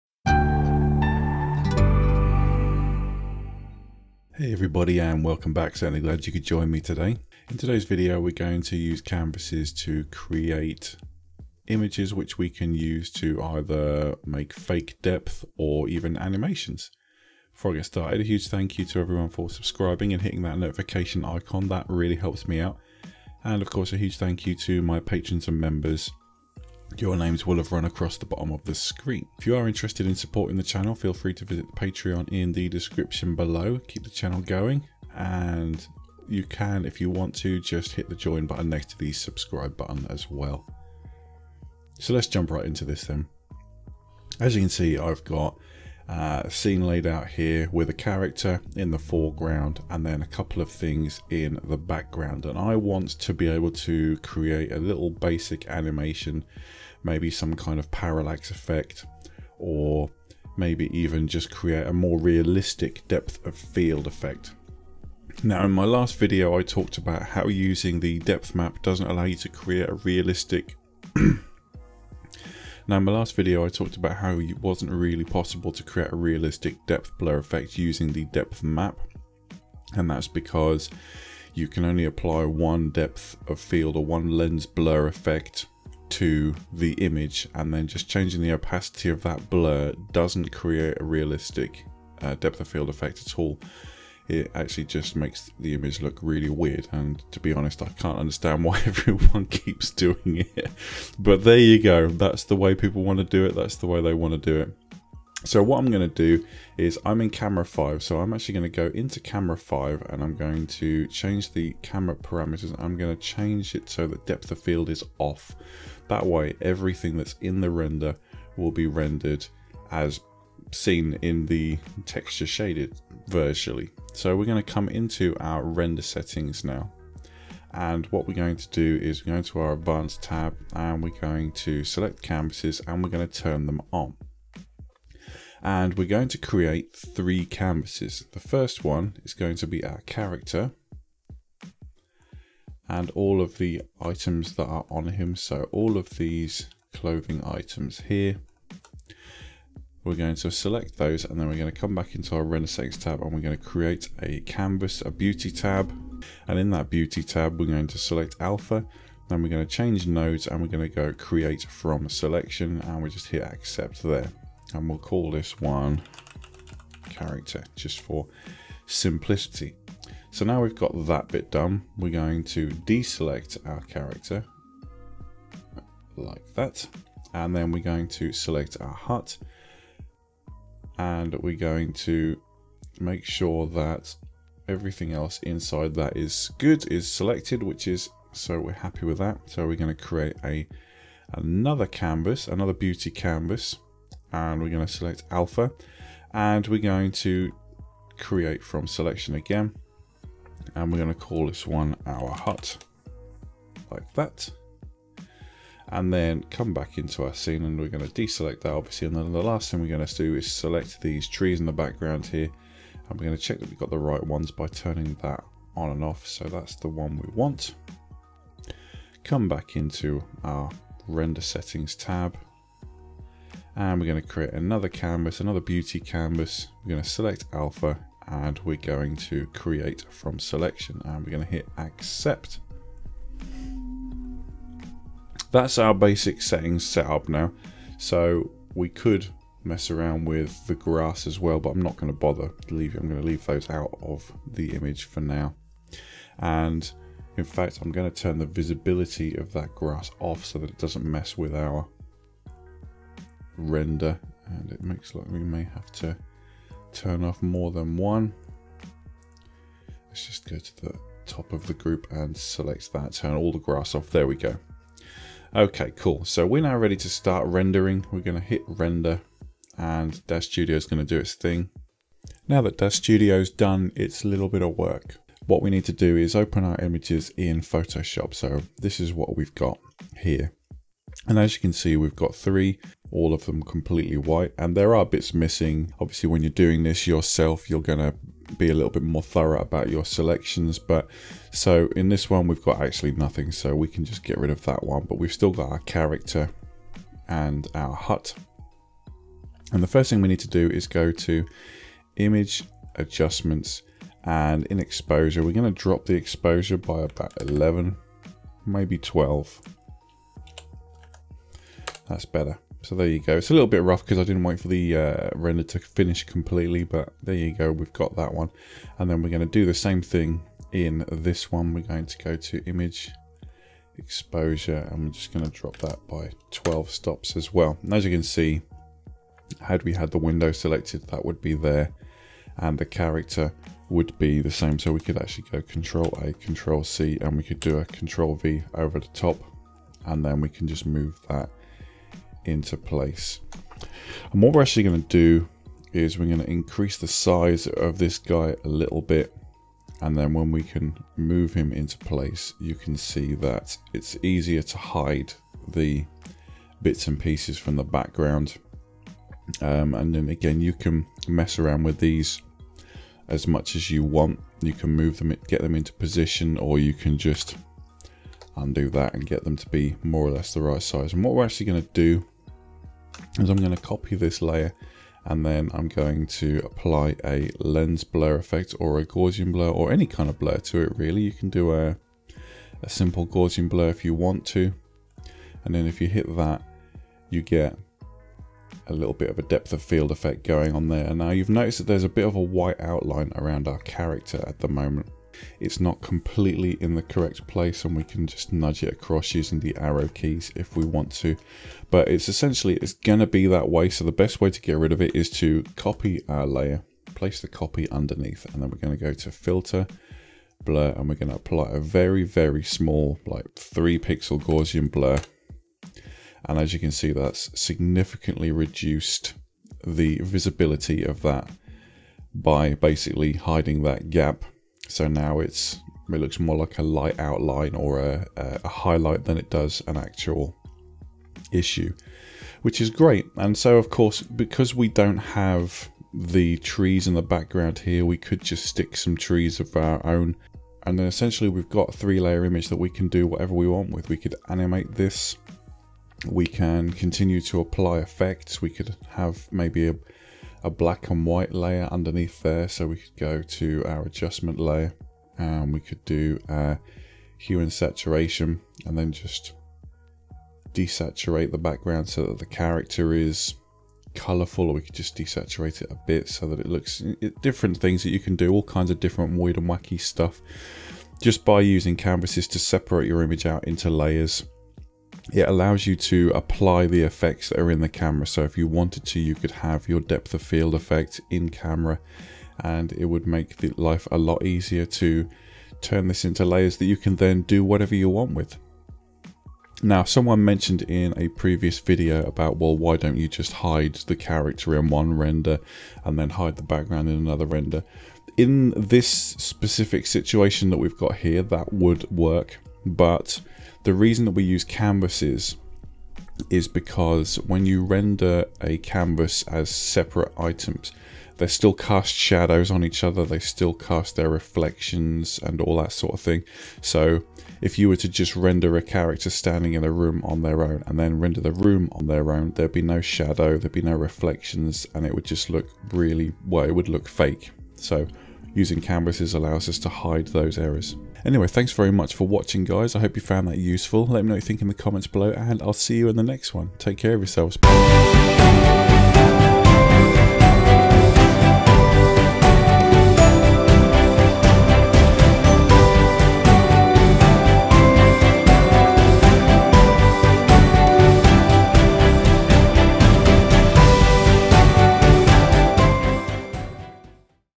Daz Studio pro Tips: Canvas - Rendering multiple layers simultaneously
In this video we look at how to render out multiple layers from the same scene at the same time to use for compositing or animation or... the list is endless

All information contained in this video is correct to the best of my knowledge at the time of recording.

Links